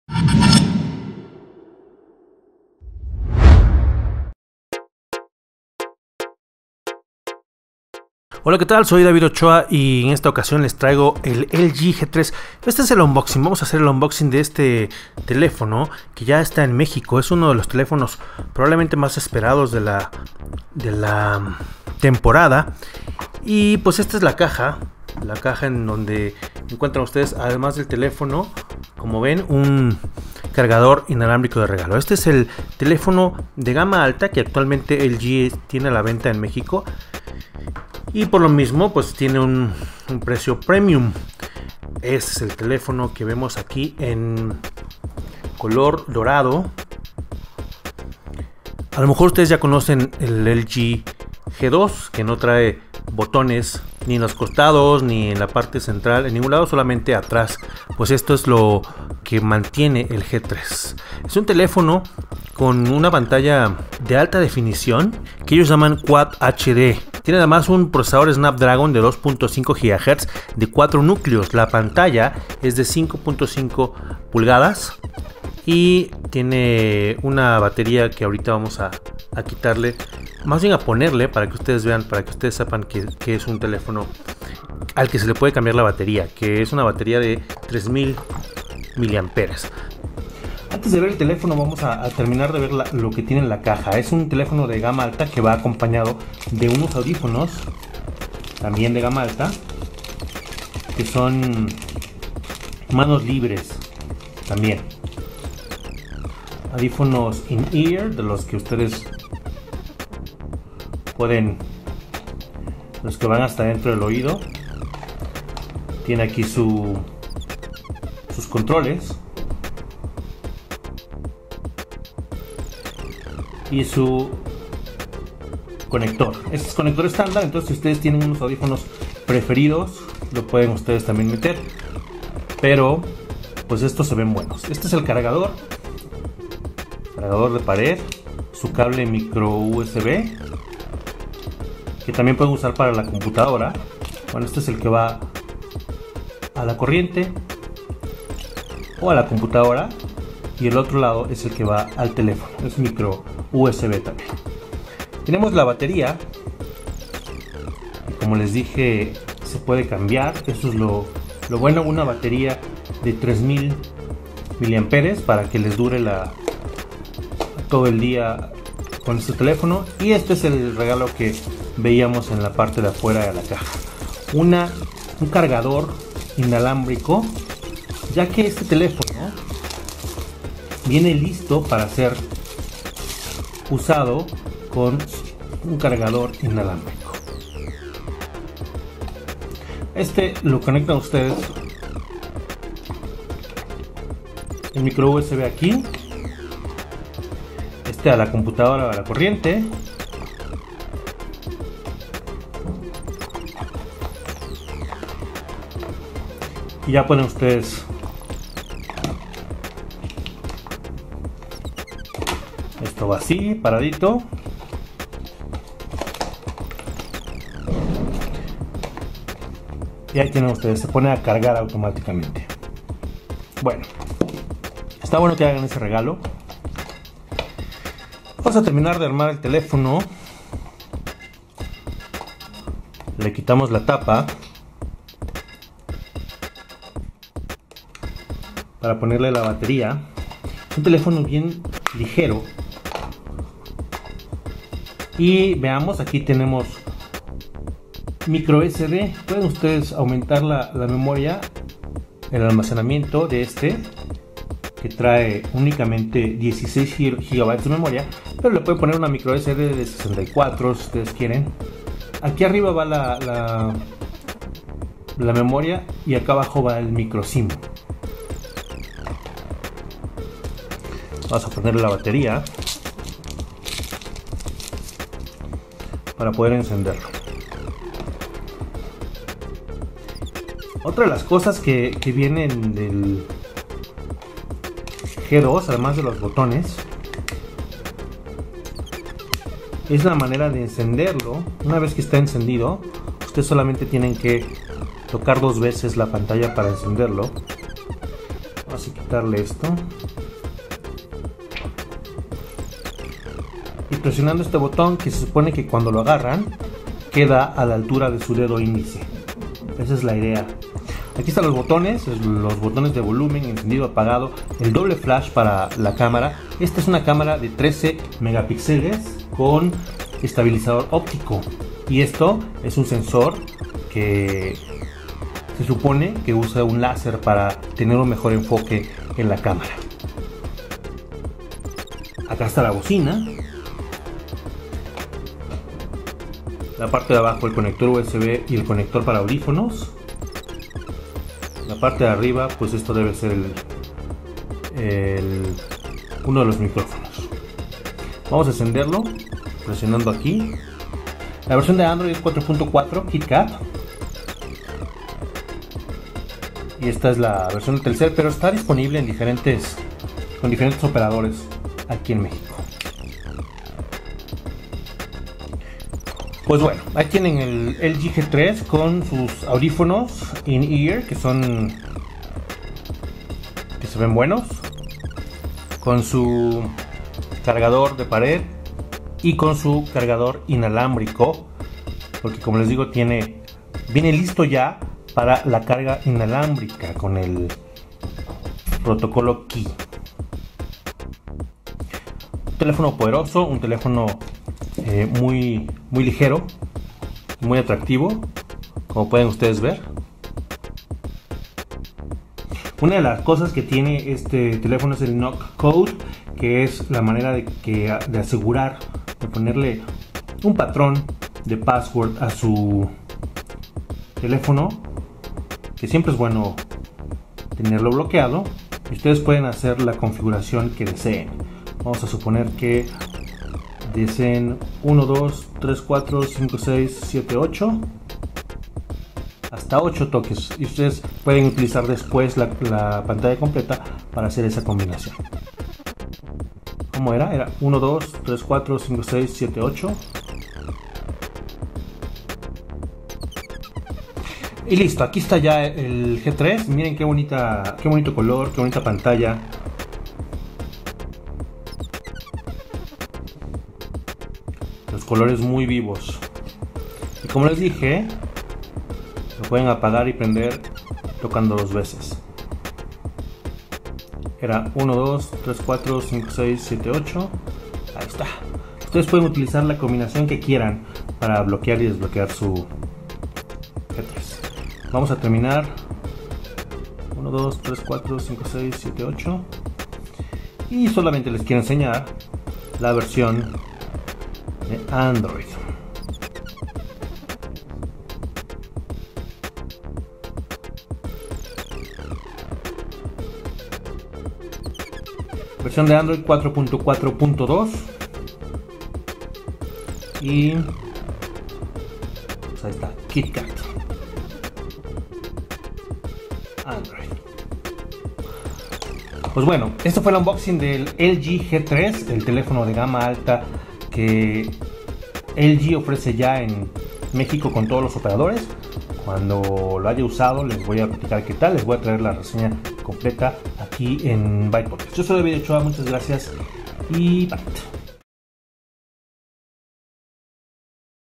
Unboxing del LG G3
Les mostramos el contenido de la caja de un smartphone LG G3 (modelo D855P) en México. El G3 tiene Android 4.4, cámara principal de 13 Mpx y pantalla de 5.5 pulgadas.